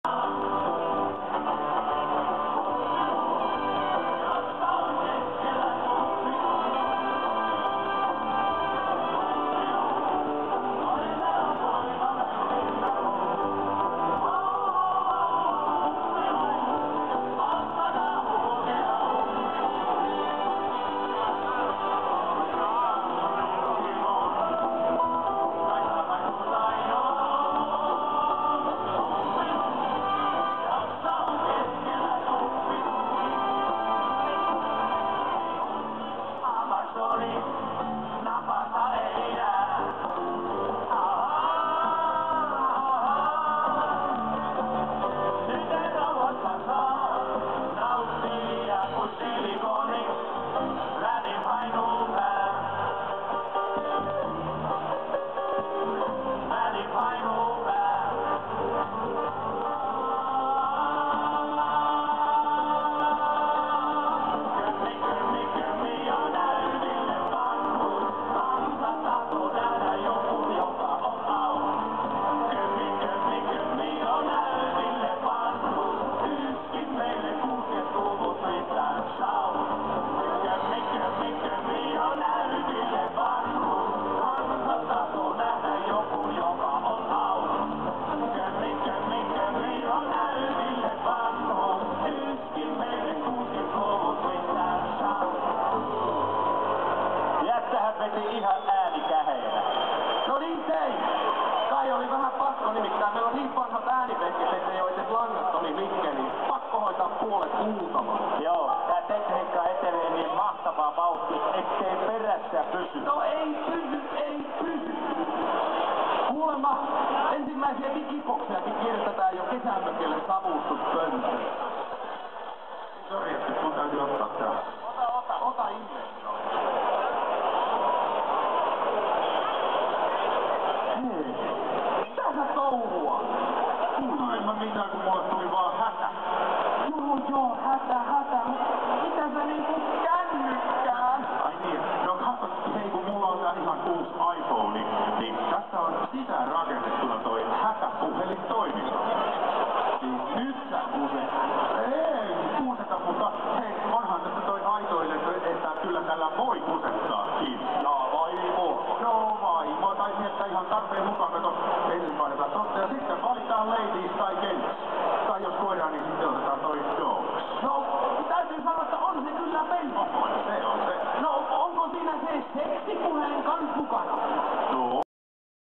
Seven Ravingmad Brothers sings ABBA in Oasis of the Seas employees jubilee
Band name is "Seitsemän Seinähullua Veljestä".

Originally acknowledged  name with  "Seven Ravingmad Brothers of Pentti Oskari Kangas".

Originally acknowledged  name with  " Pentti Oskari Kankaan Seitsemän Seinähullua Veljestä".

Famous entertaining, humour group in Finland.